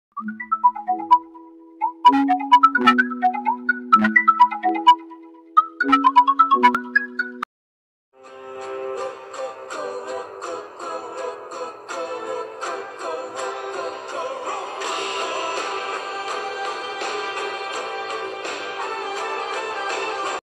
Any Animation Meme requests you guys have for me?
It's not like I'm running out of ideas on what to upload, I'm just taking a break from uploading GD content.